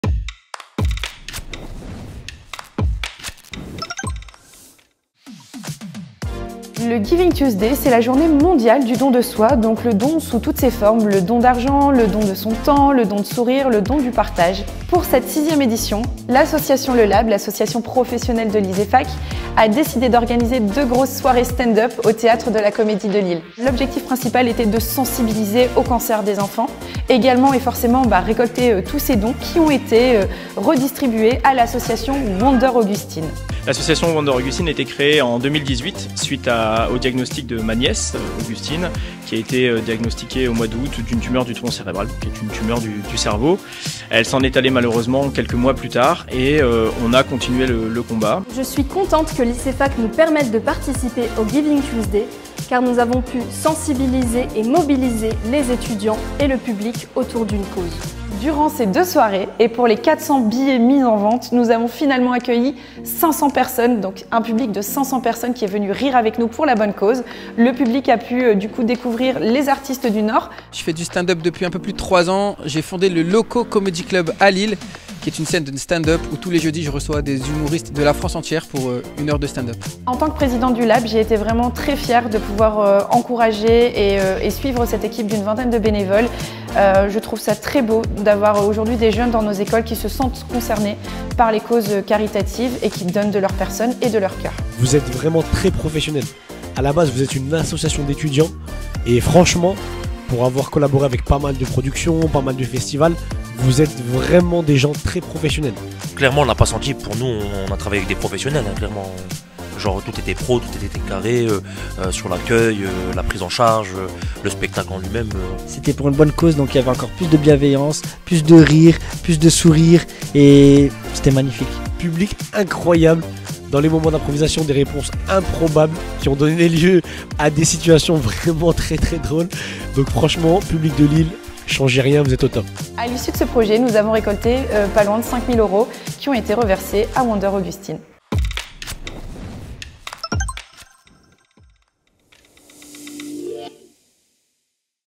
Aftermovie : Giving Tuesday 2022
Chaque année, l’association professionnelle le Lab, figure emblématique de l'ISEFAC de Lille, renouvelle son engagement pour le Giving Tuesday, la journée mondiale dédiée à la générosité et à la mobilisation sociale.

Découvrez en images l'aftermovie du Giving Tuesday 2022 et des deux soirées organisées par les étudiants au profit de l'association Wonder Augustine !